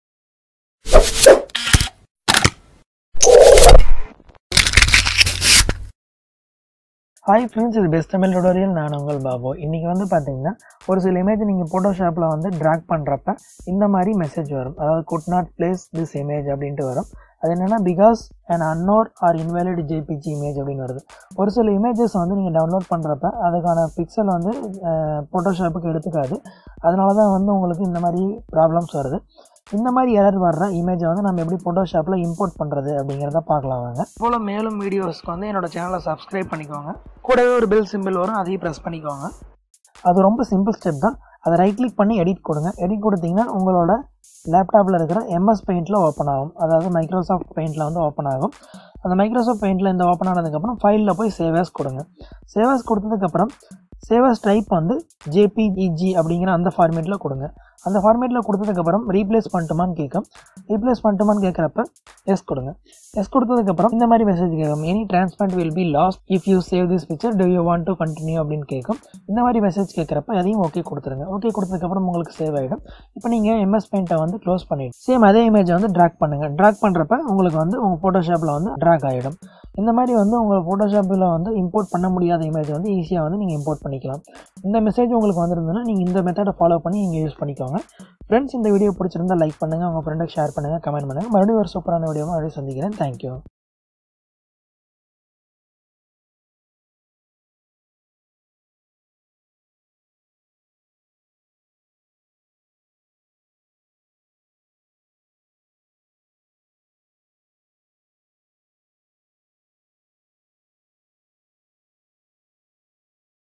HOW TO FIX COULD NOT PLACE THIS IMAGE BECAUSE AN UNKNOWN JPEG MARKER TYPE IS FOUND - BTT
How to Fix Could not Place this image Because an Unknown or Invalid JPEG Marker type is found